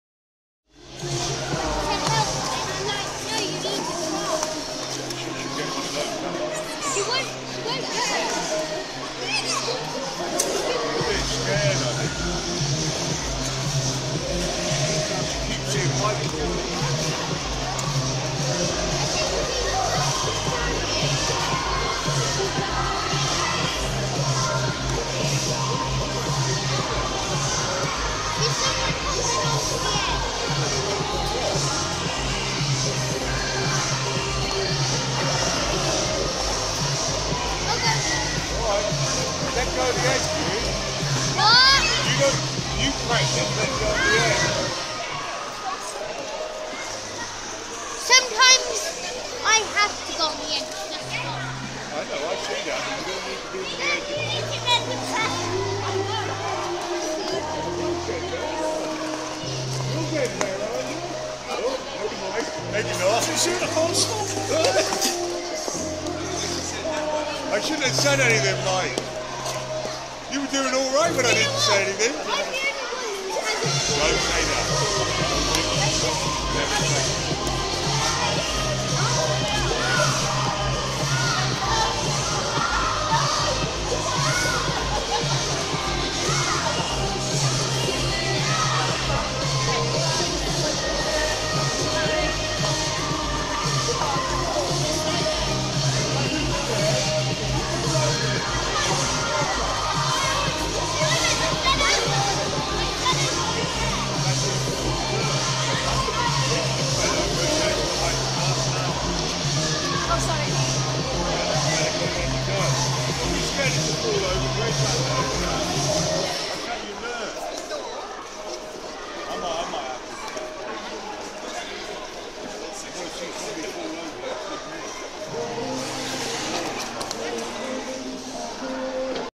Początki na lodzie
Tata z trójka dzieci na lodowisku